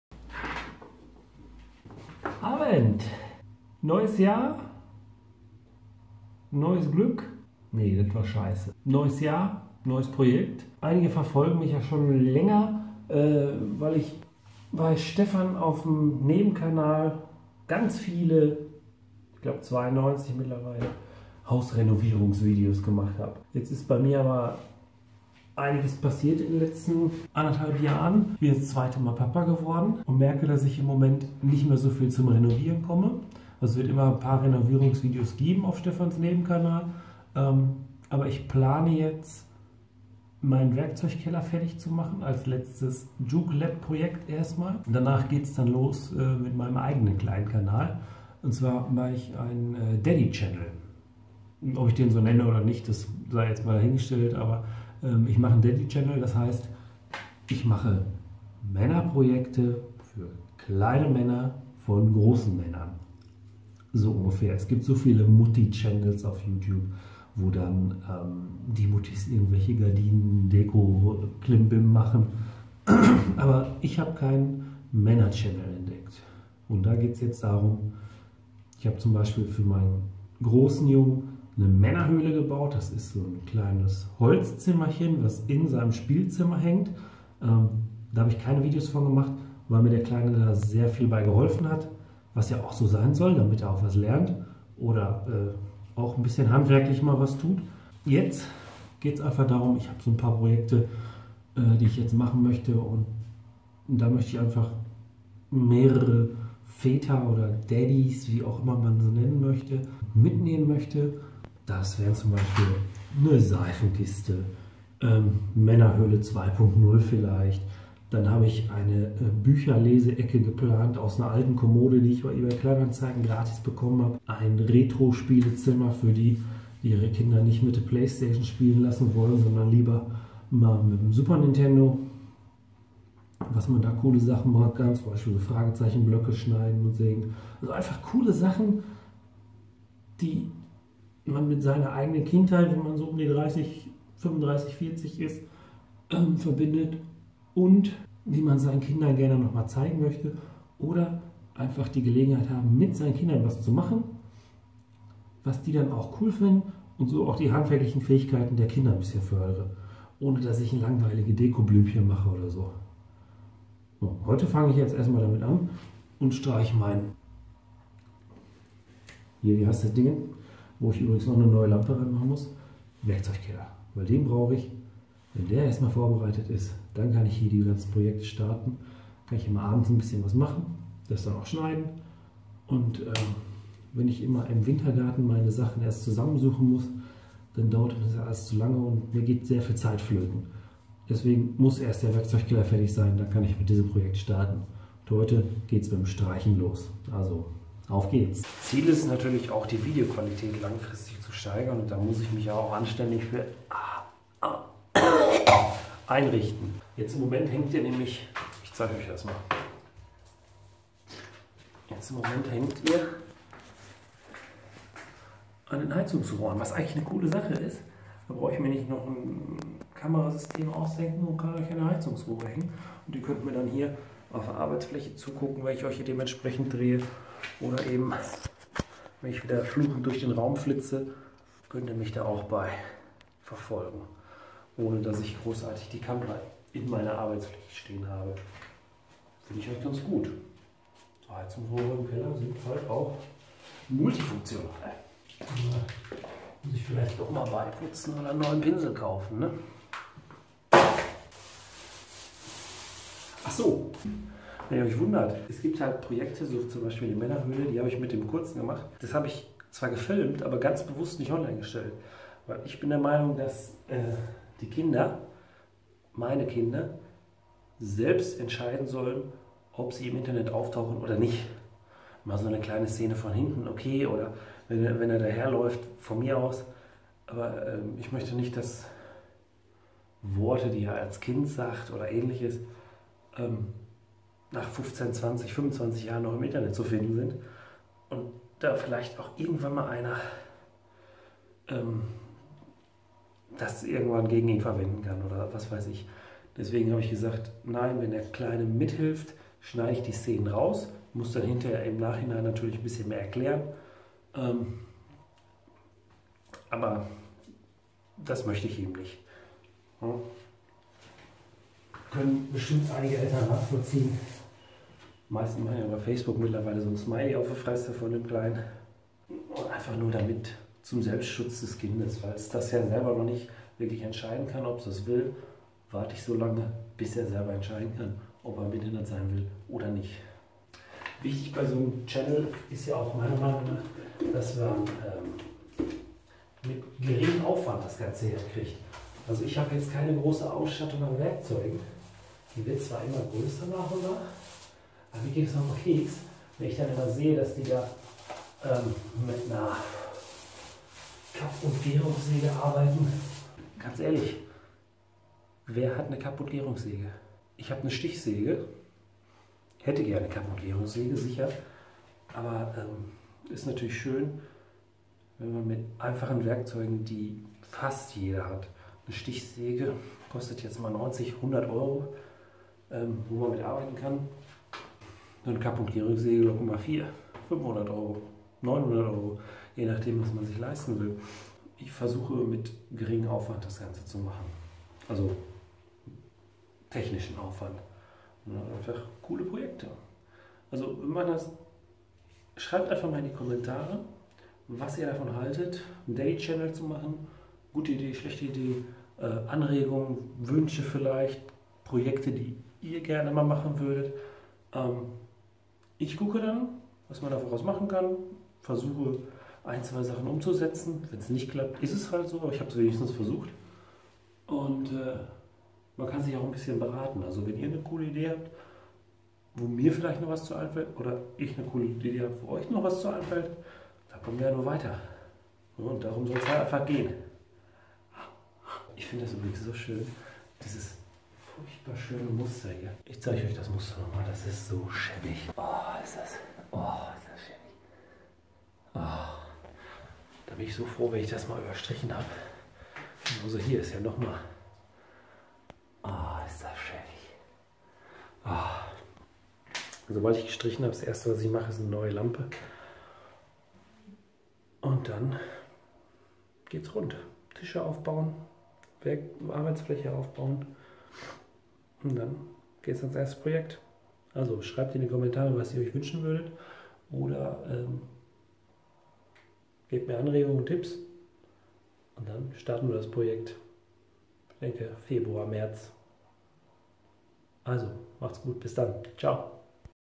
Neues Jahr, neues Projekt - Was haltet ihr davon...?!
Hey,
bei Christian ist im letzten Jahr so einiges  passiert, er hat neue Ideen und brauch einmal eure Meinung dazu. Nebenbei wird auch noch ein bisschen gestrichen.

Equipment zum Vloggen:

◘ Canon Powershot G7x Mark 2
◘ Manfrotto Pixi

Allerdings habt Ihr dadurch die Möglichkeit uns zu Unterstützen